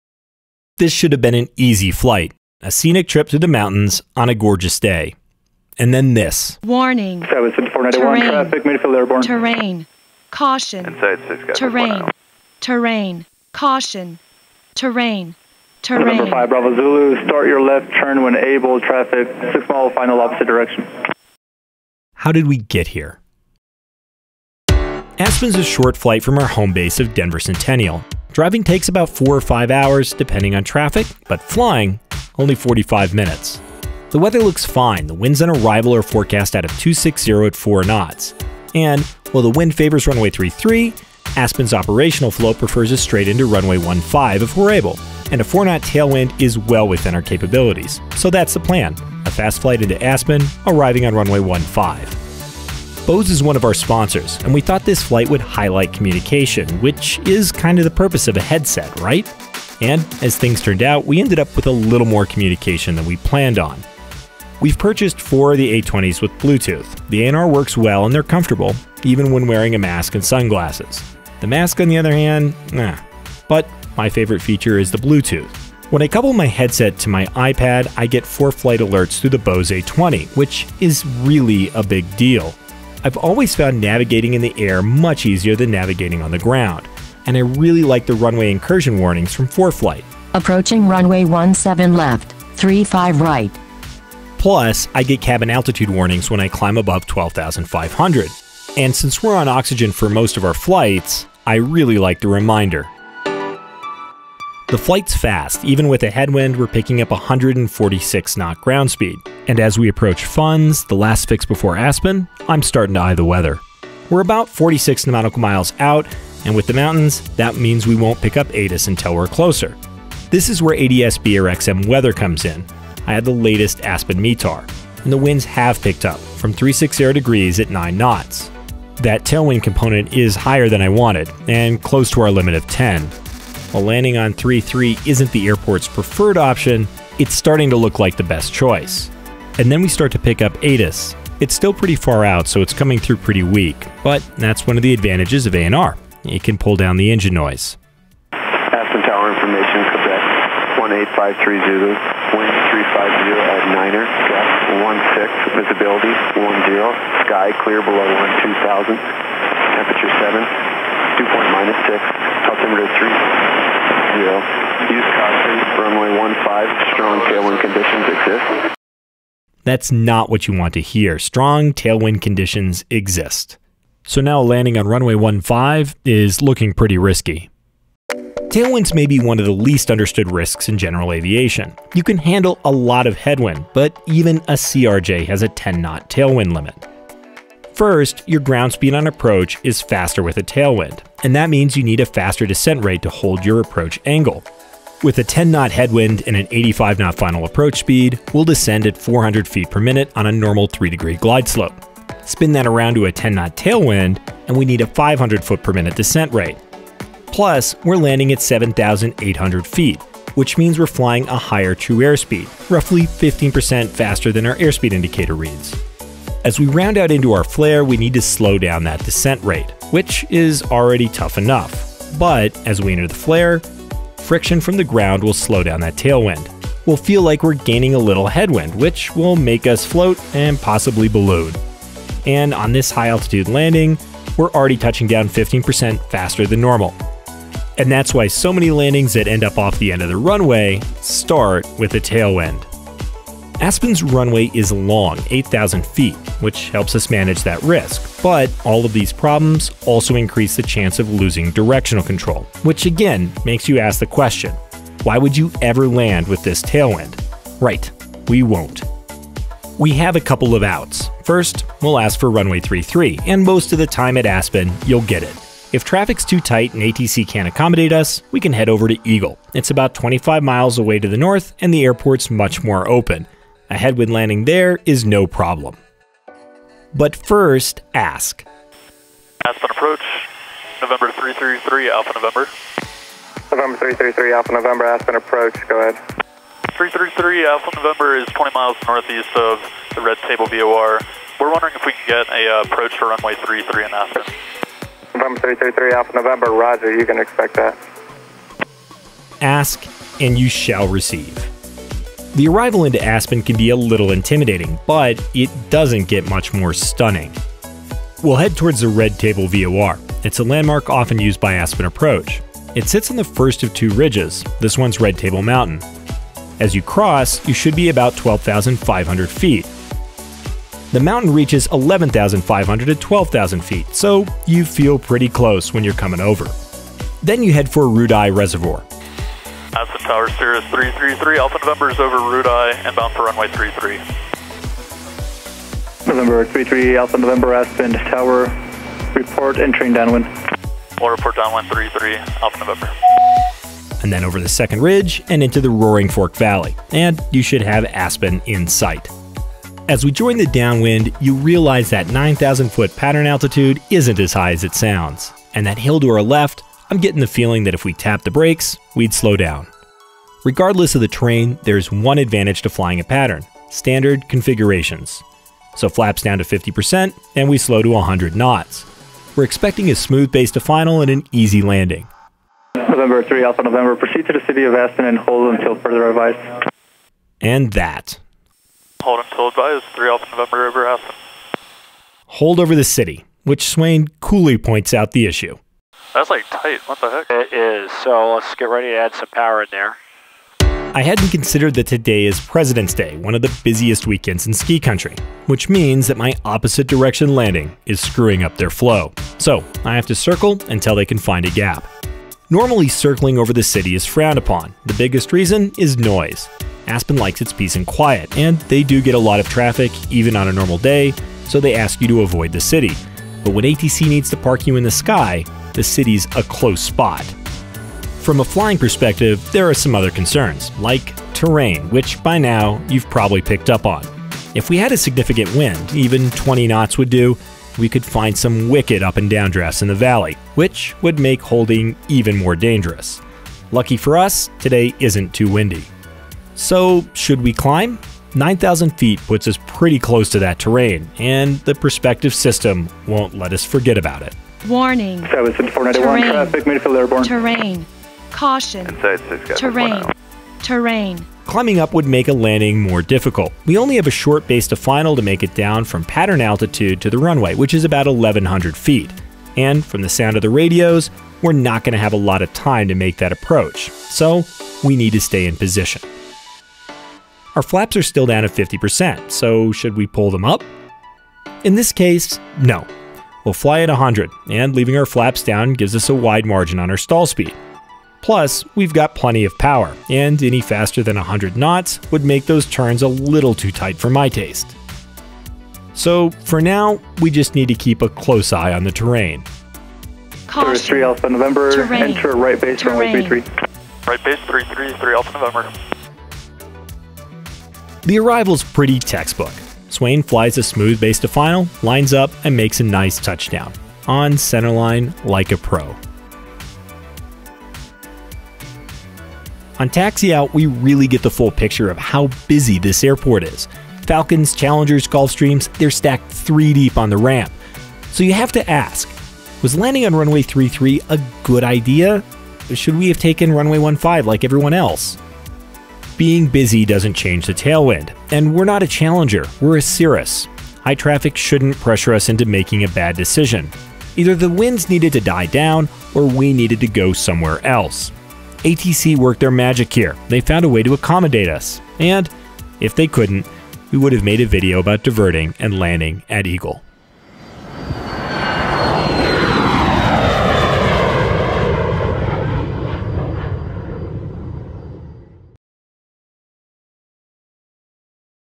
Landing In Aspen With A 16 Knot Tailwind
Should we land with a tailwind, hold over a narrow canyon, or divert? On this flight, communication was the key to getting into Aspen in one piece.

Bose sponsored this video - without them, it wouldn't have been possible.